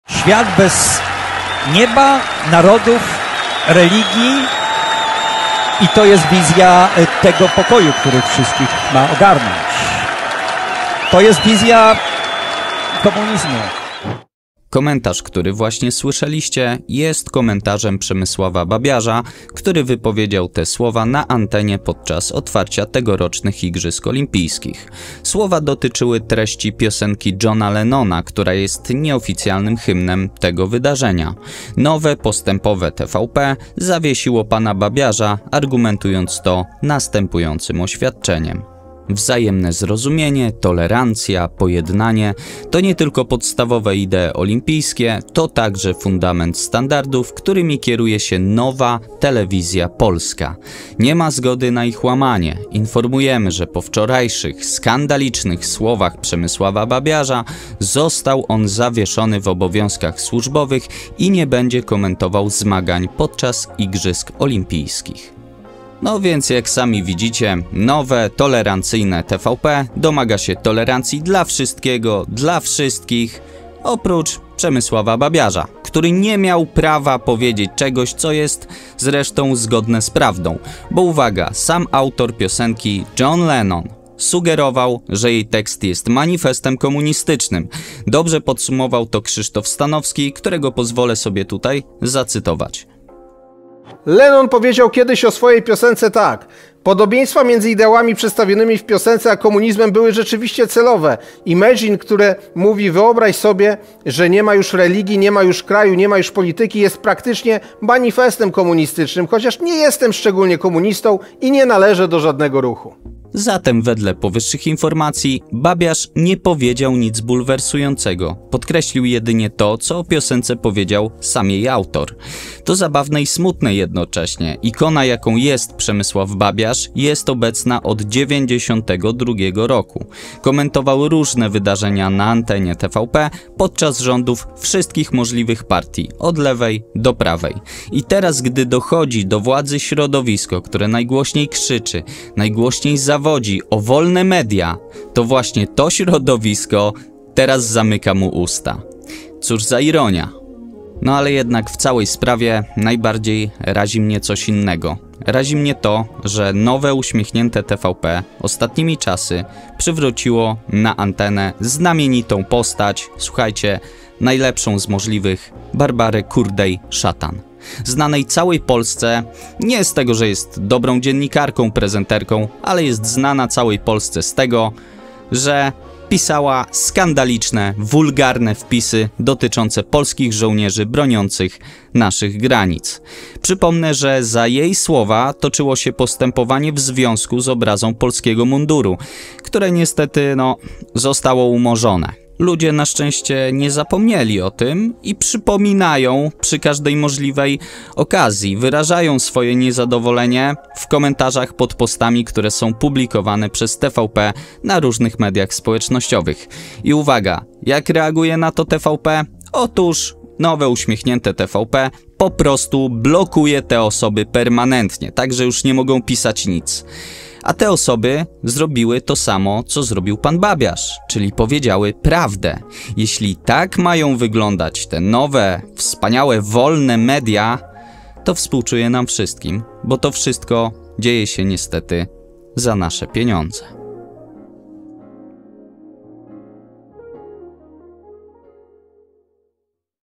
BABIARZ ZAWIESZONY A KURDEJ-SZATAN POWRACA... WOLNE MEDIA (mój komentarz)
Przemysław Babiarz został zawieszony po swoich słowach podczas transmisji otwarcia Igrzysk, a Kurdej-Szatan powraca... Nowe TVP.

Jeżeli podoba Ci się to co robię możesz mnie wesprzeć poniżej :)
lub: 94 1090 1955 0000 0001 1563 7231
tytułem "DAROWIZNA"
DZIĘKUJĘ!